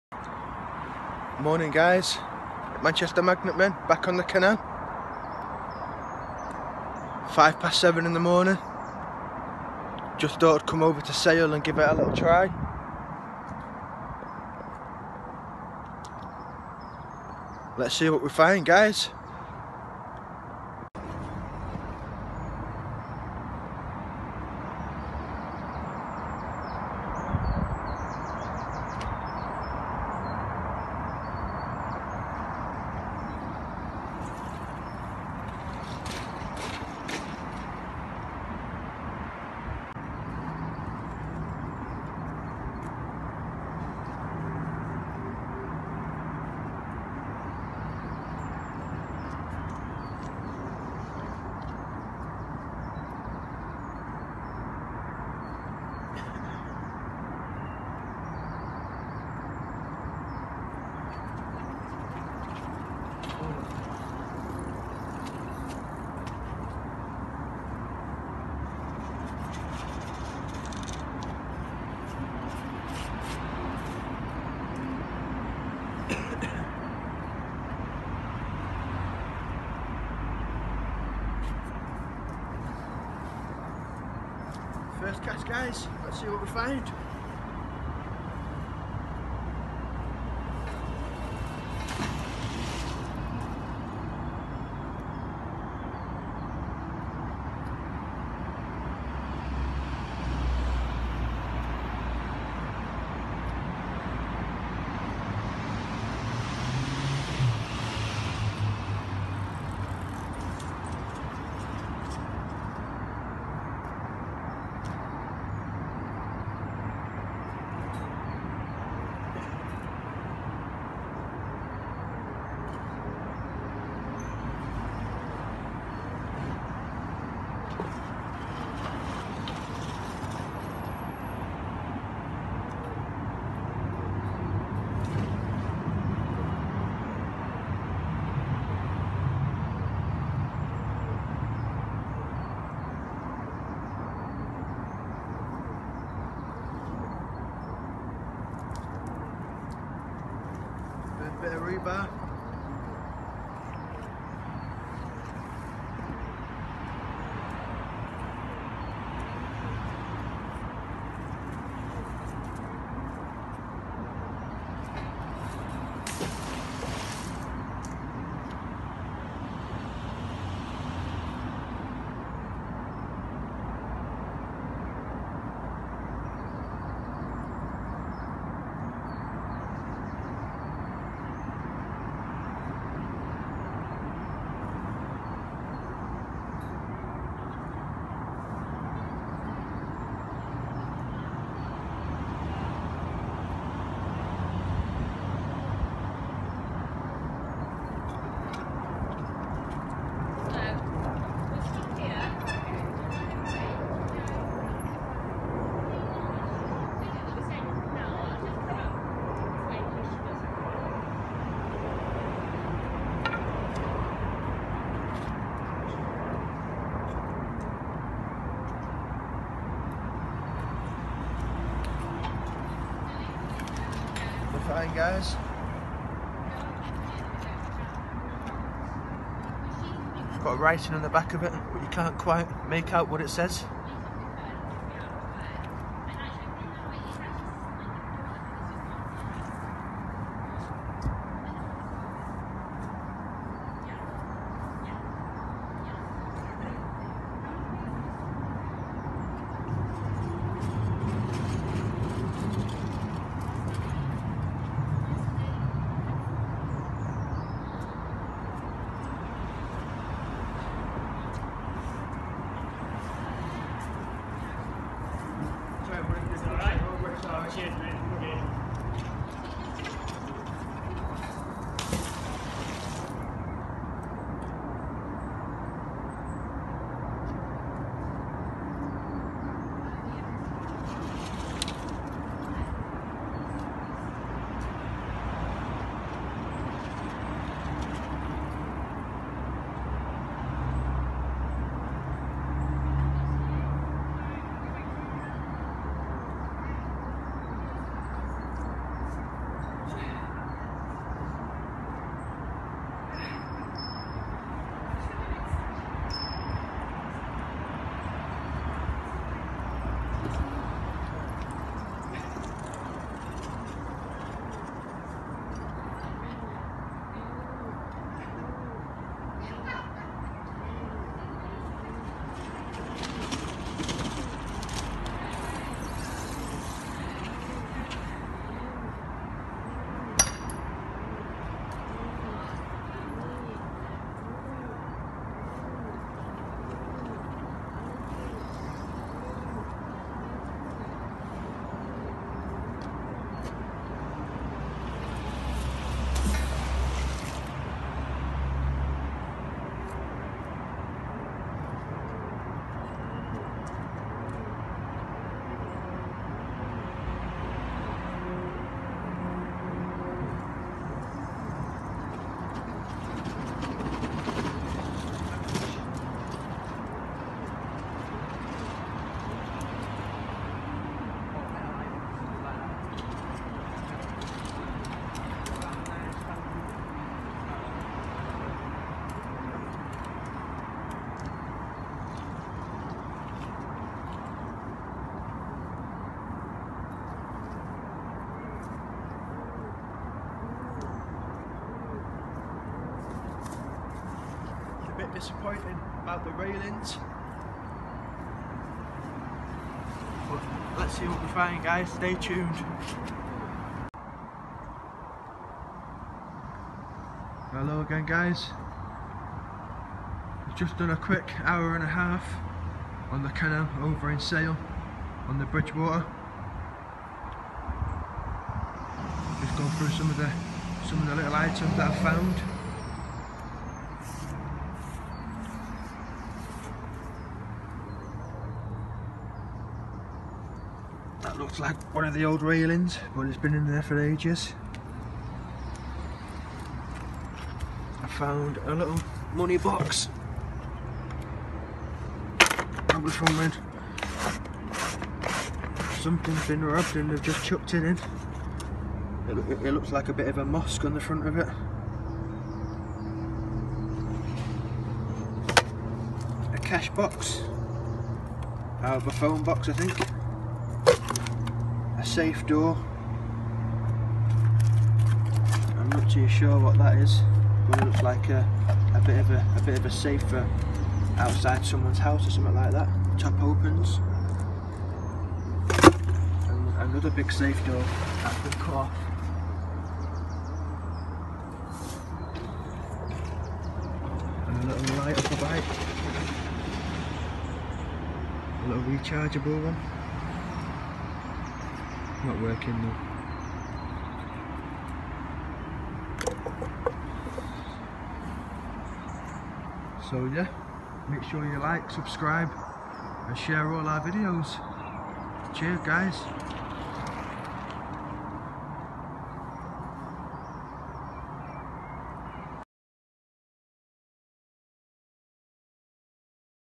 Magnet fishing with Manchester magnet men
Magnet fishing with Manchester magnet men in sale the other day. Some cool finds Jason found that day.